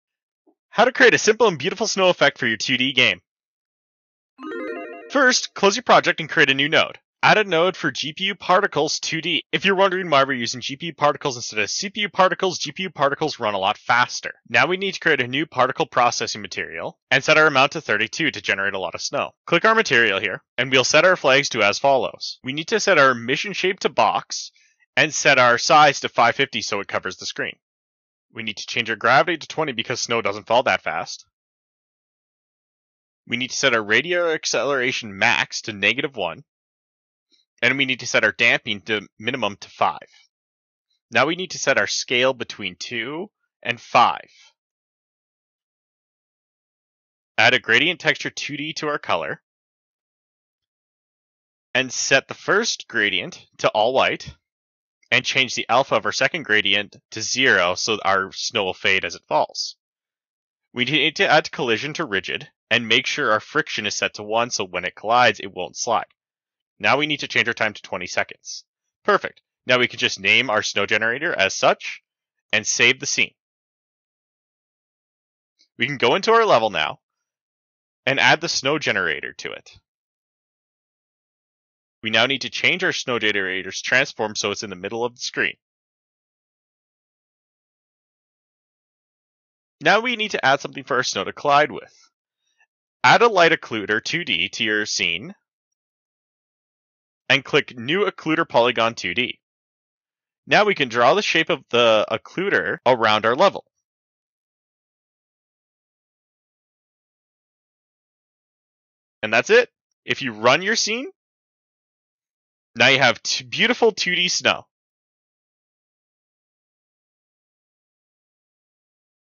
Make 2D Snow Particle Effect In Godot In 2 Minutes
In this video we will make a easy and beautiful 2d snow in Godot 4